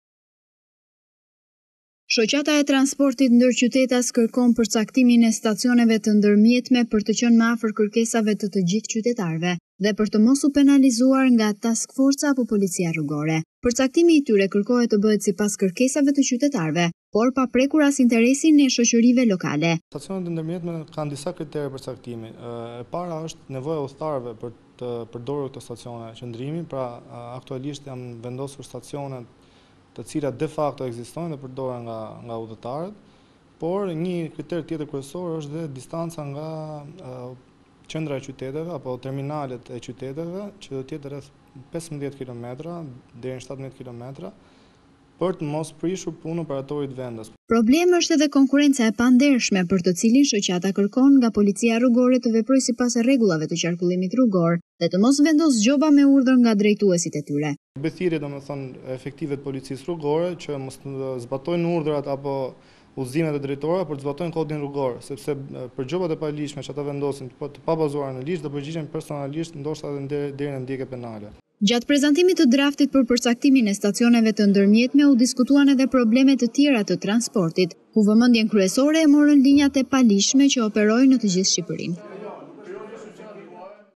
Ora News - Autobusët kërkojnë ndalesa
Shoqata e Transportit Ndërqytetas kërkon nga Ministria e linjës përcaktimin e stacioneve të ndërmjetme. Autobusët e linjave ndërqytetase kërkojnë të ndalojnë në bazë të kërkesave të qytetarëve, por të shmangur gjobat e policisë dhe të task-forcës.

Shoqata e Transportit Ndërqytetas kërkon përcaktimin e stacioneve të ndërmjetme për të qenë më afër kërkesave të të gjithë qytetarëve dhe për të mos u penalizuar nga task-forca apo policia rrugore.

Përcaktimi i tyre kërkohet të bëhet sipas kërkesave të qytetarëve por pa prekur as interesin e shoqërive lokale.

Eneo Sulejmani: Stacionet e ndërmjetme kanë disa kritere përcaktimi. E para është nevoja e udhëtarëve për të përdorur këto stacione qëndrimi, aktualisht janë vendosur stacione që de-facto ekzistojnë dhe përdoren nga udhëtarët, por një kriter tjetër kryesor është dhe distanca nga qendra e qyteteve apo terminalet e qyteteve që do të jetë rreth 15-17 km për të mos i prishur punë operatorit vendas.